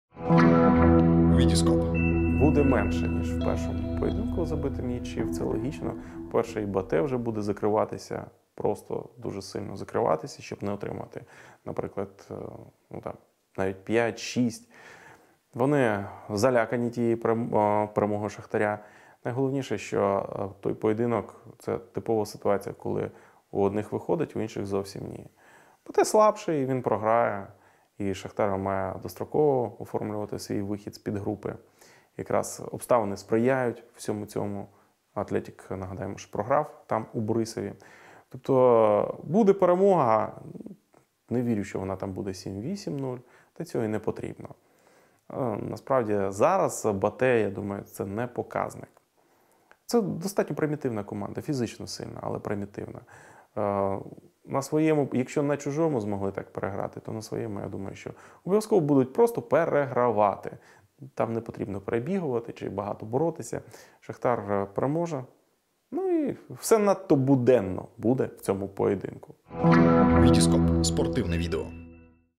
Роберто Моралес про матч Шахтар – БАТЕ
Футбольний комментатор Роберто Моралес вважає, що в повторному матчі Ліги чемпіонів Шахтар - БАТЕ нових рекордів результативності встановлено не буде.

Буде менше ніж у першому поєдинку забито м’ячів, це логічно. По-перше, і БАТЕ буде більше закриватися, просто буде дуже сильно закриватися, щоб не отримати, наприклад, п’ять-шість. Вони дуже залякані тією перемогою Шахтаря. Найголовніше, що той поєдинок це типова ситуація коли у одних виходить, у інших зовсім нічого не виходить.

БАТЕ слабше, він програє і Шахтар має достроково оформлювати свій вихід з підгрупи, якраз обставини сприяють цьому. Нагадаємо, Атлетік там програв у Борисові, тобто буде перемога. Не вірю що вона буде 7 або 8:0. Не потрібно. Насправді, зараз БАТЕ — це не показник, це достатньо примітивна команда, фізично сильна, але примітивна. Якщо на чужому змогли так переграти, то на своєму обов’язково просто будуть перегравати. Там не потрібно перебігувати чи багато боротися. Шахтар переможе, все надто буденно буде в цьому поєдинку.